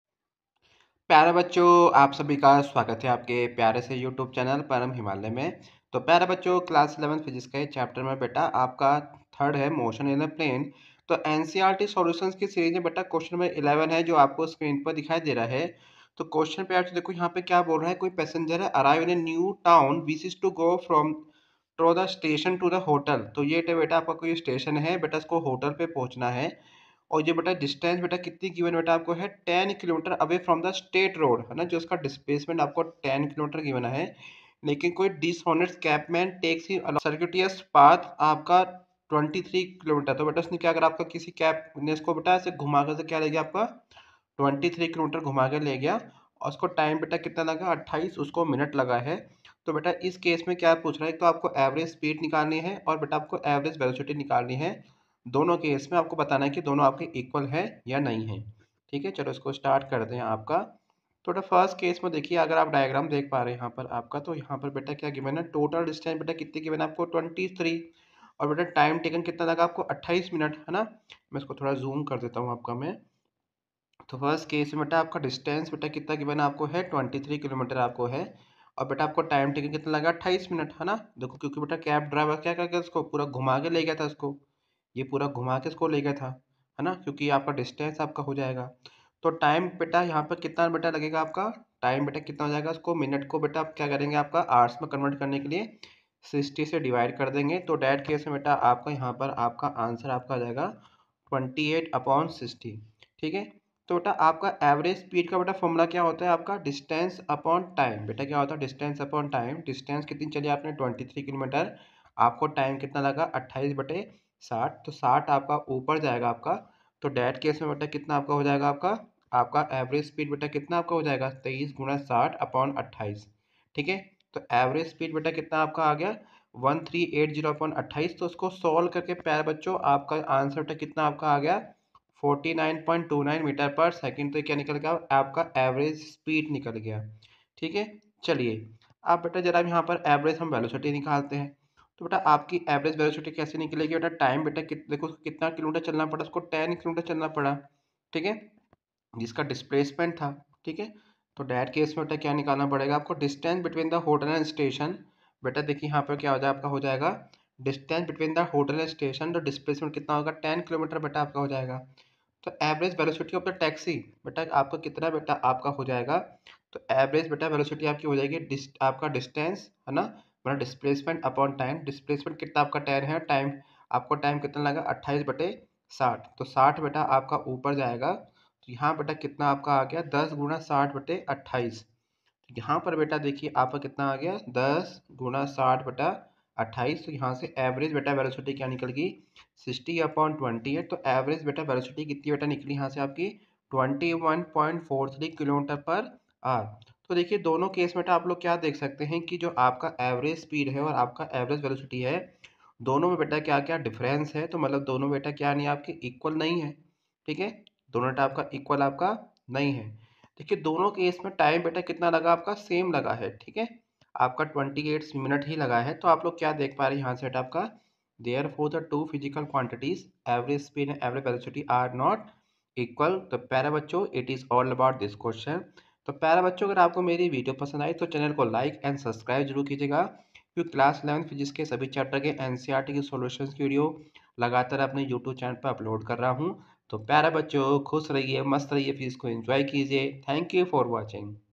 3.11 A passenger arriving in a new town wishes to go from the station to a hotel located 10 km away
3.11 A passenger arriving in a new town wishes to go from the station to a hotel located 10 km away on a straight road from the station. A dishonest cabman takes him along a circuitous path 23 km long and reaches the hotel in 28 min. What is (a) the average speed of the taxi, (b) the magnitude of average velocity ? Are the two equal ?

Ncert Solution Class 11 Physics Chapter 3 motion in plane